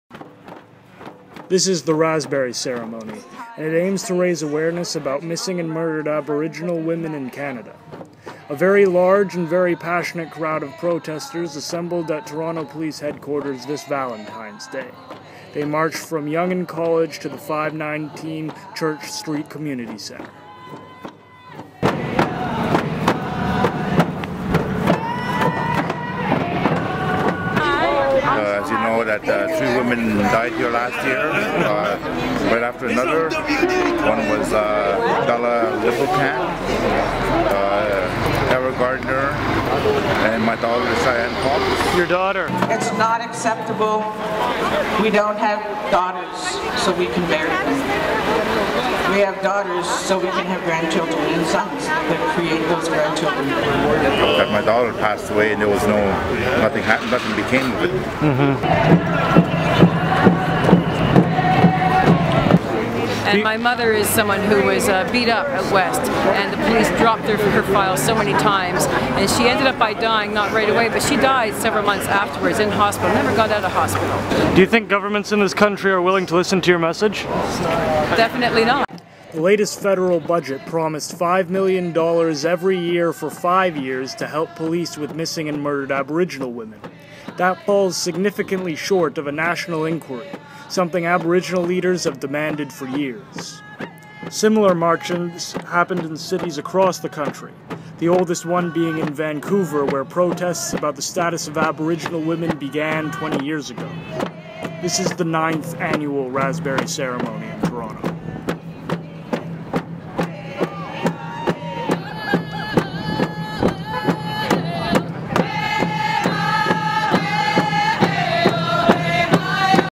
Strawberry Ceremony for Missing and Murdered Aboriginal Women
One day after a Halifax university student, Loretta Saunders went missing, hundreds of demonstrators in Toronto held the annual Strawberry Ceremony in honour of the missing and murdered Aboriginal women in Canada.

John Fox, Ontario's Grass Roots Committee Chair and father of recently deceased Cheyenne Fox, led the march from Toronto Police Headquarters to the 519 Church Street Community Centre.